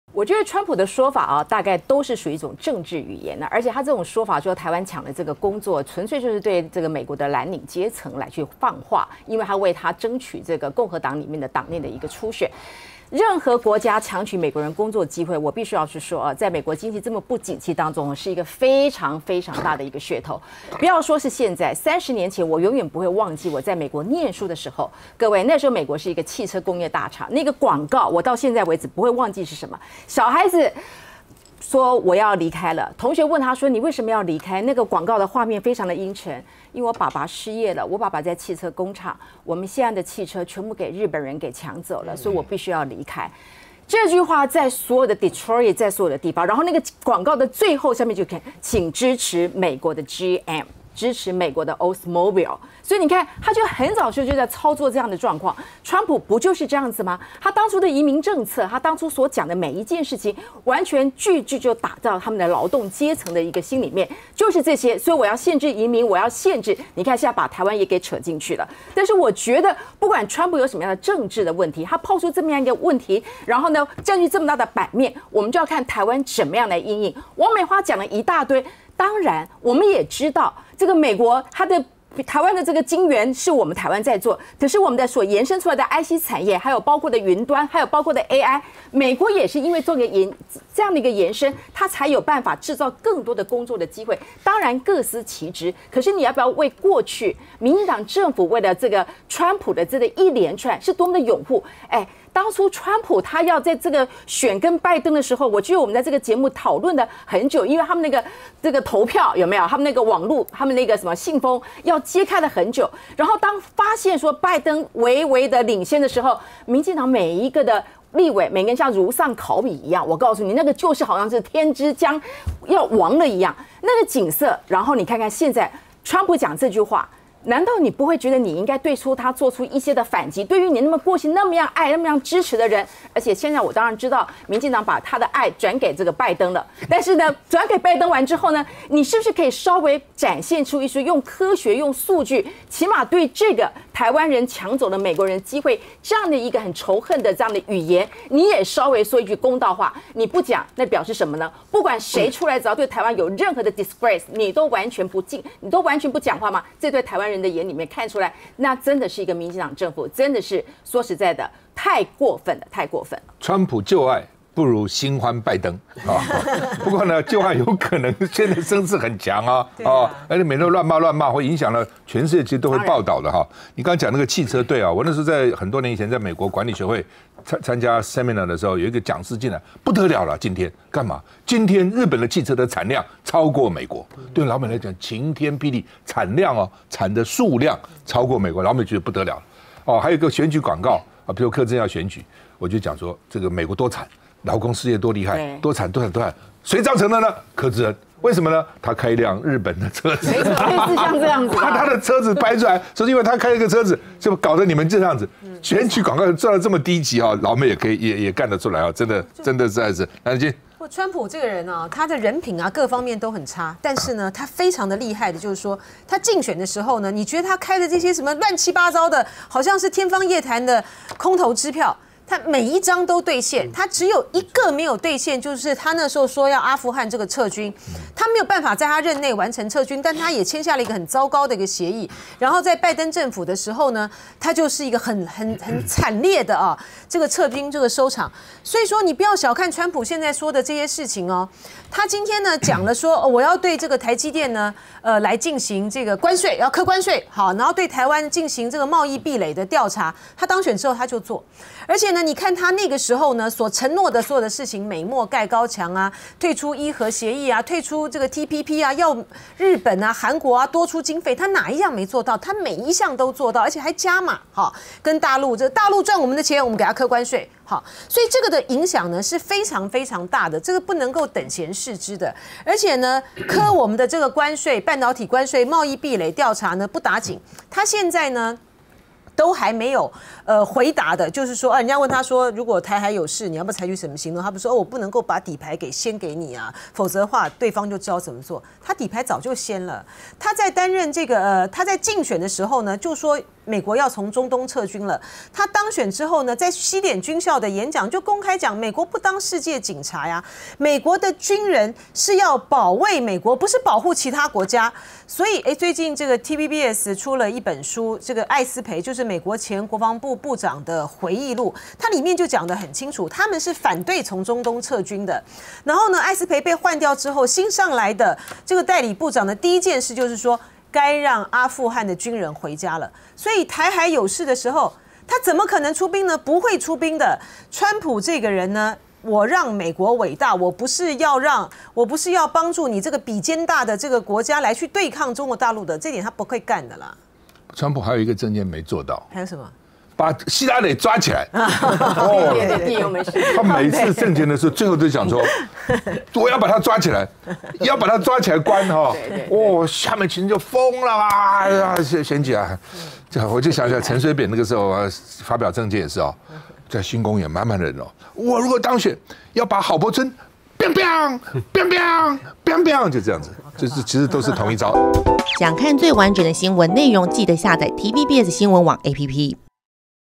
【今日精華搶先看】專家:過河拆橋欠公允 民進黨對川普恐入主白宮只愛不恨?
本錄影遵守防疫規範。